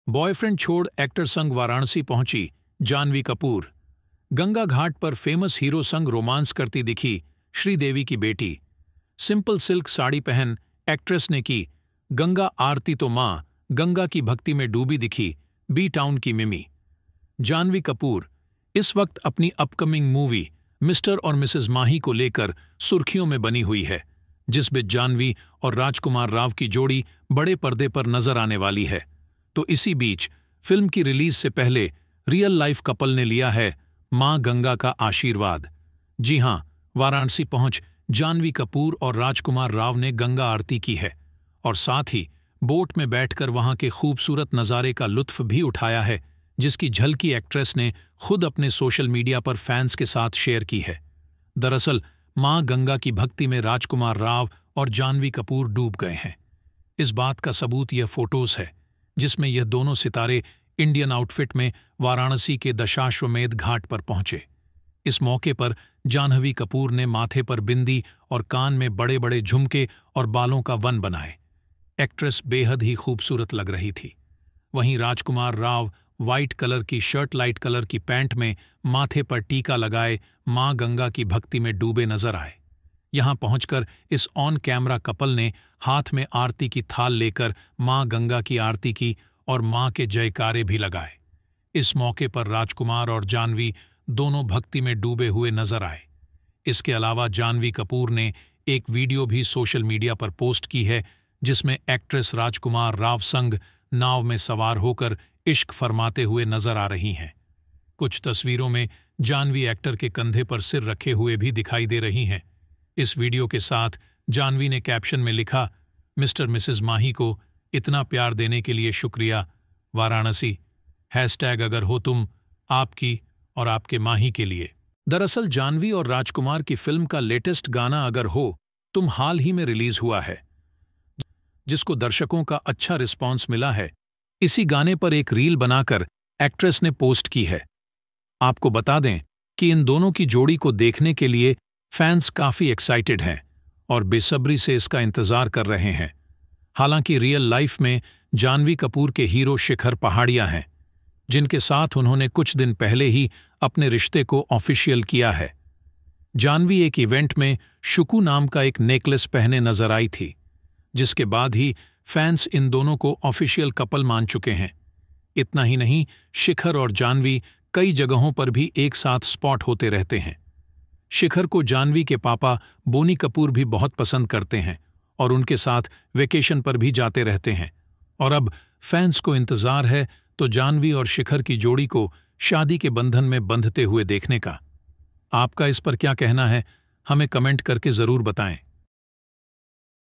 Janhvi Kapoor और Raj Kummar Rao पहुंचे काशी, माँ गंगा की आरती में हुई शामिल
Janhvi Kapoor और Raj Kummar Rao पहुंचे काशी, माँ गंगा की आरती में हुई शामिल | Janhvi Kapoor And Raj Kummar Rao Performed Ganga Aarti On The Ghat At Varanasi | Janhvi Kapoor | Raj Kummar Rao |